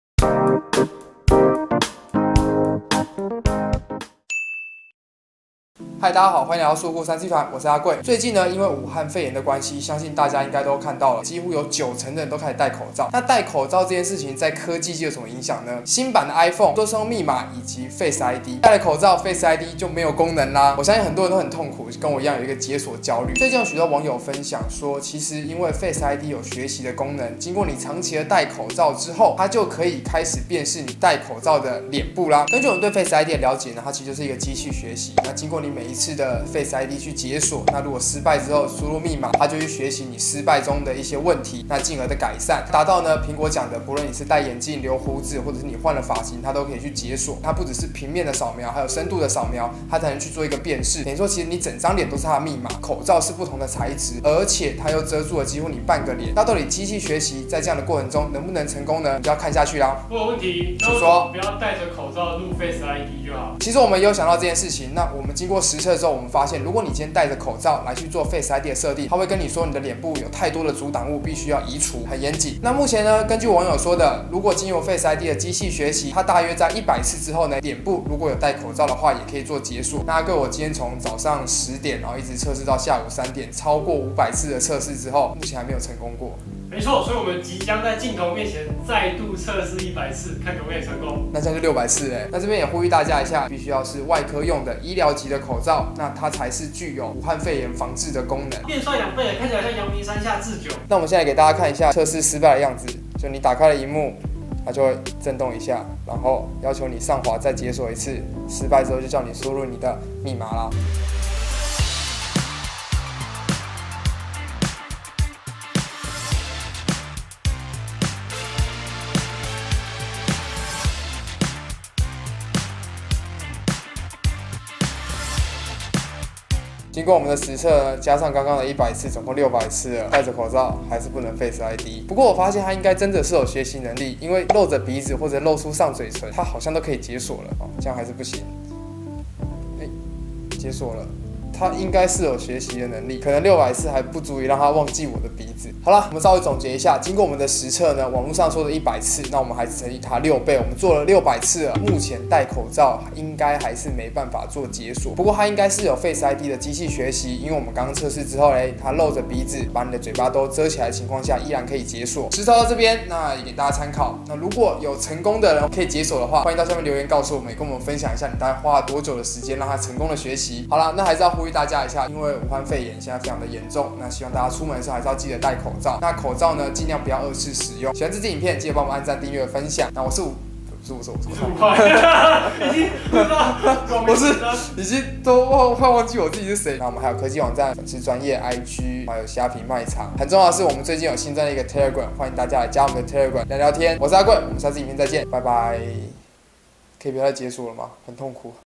戴口罩可以解鎖iPhone嗎？訓練600次Face ID【束褲科技】
人人戴上口罩自保
這時候iPhone就沒有辦法臉部辨識了
怎麼辦？
網路上說可以訓練Face ID
是真的還假的？

手機挑戰市場最低價
再送千元尊榮服務卡

『好影片，不看ㄇ？』
【束褲開箱】